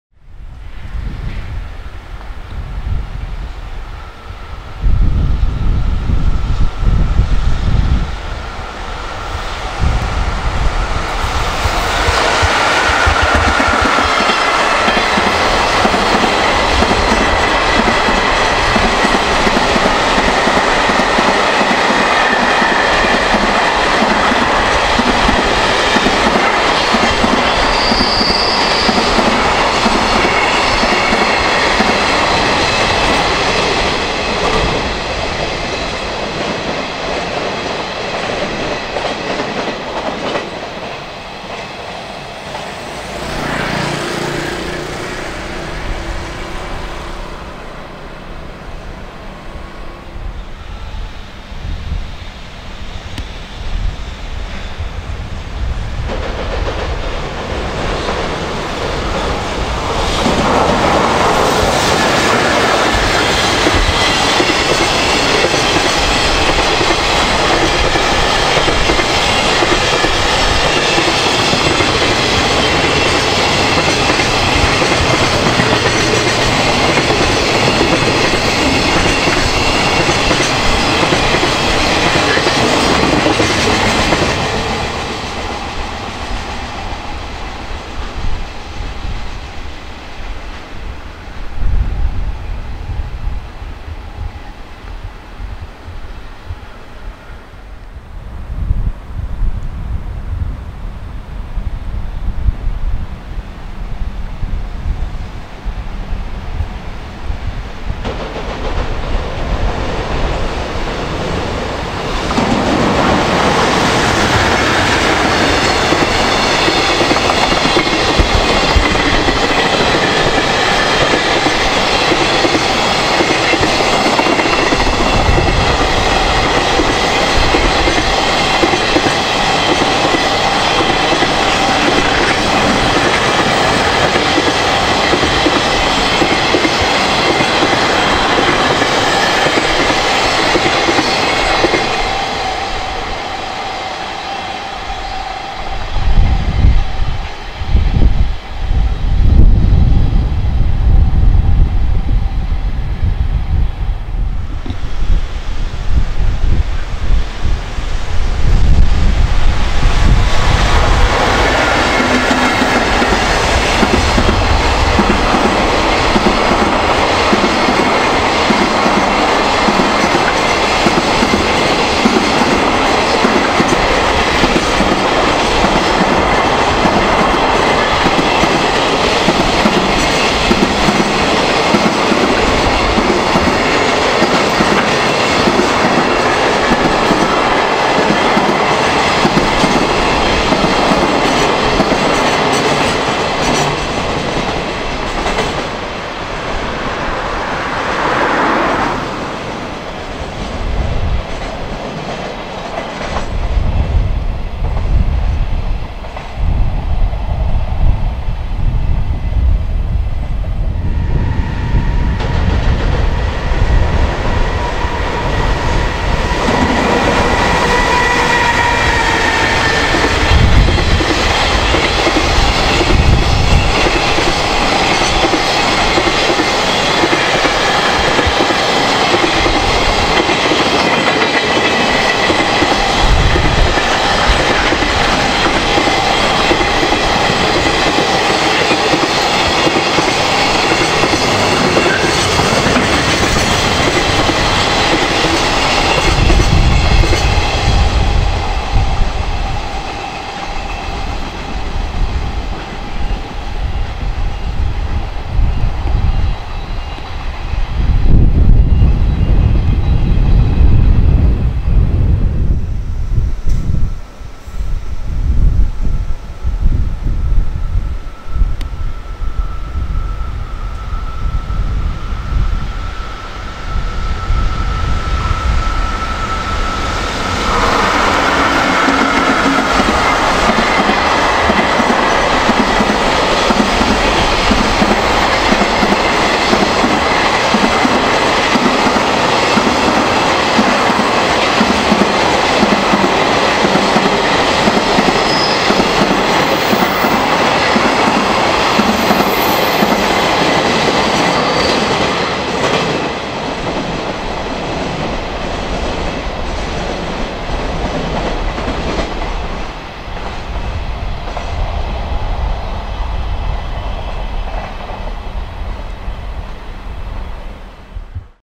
2015/11/09 東海道本線 藤枝駅 貨物6本
2015/11/09 東海道本線 藤枝駅
15:36 EF66-119
16:07 EF210-143
16:26 EF210-7
16:27 EF210-128
16:36 EF210-3
16:55 EF64-1011